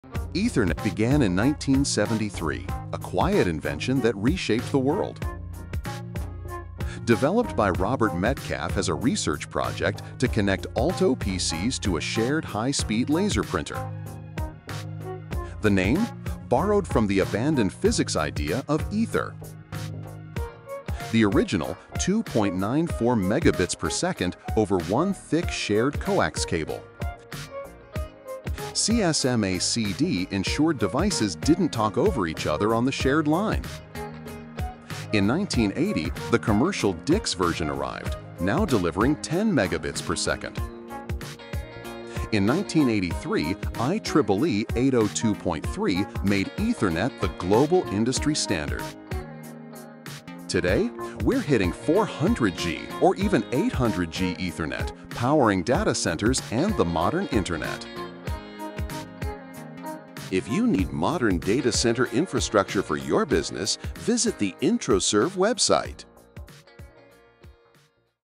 Ethernet: The Tech That Wired the World
Ethernet didn’t just connect office computers – it connected the world. Born in 1973 as a research experiment, it rapidly evolved into a global standard and the backbone of modern networking. From the first 2.94 Mbps cable to today’s 400G – 800G ultra-high-speed links, Ethernet’s story is the story of the digital age itself.

And if you need hosting with reliable connectivity up to 50 Gbps – check out introserv.com

Modifications: Image scaled (cropped) to fit the video
Slide 2
Slide 3
Original image: Living Computer Museum Seattle Visit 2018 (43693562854).jpg Author: Jason Scott Source
Modifications: Image scaled (cropped) to fit the video, the trademark was blurred
Slide 4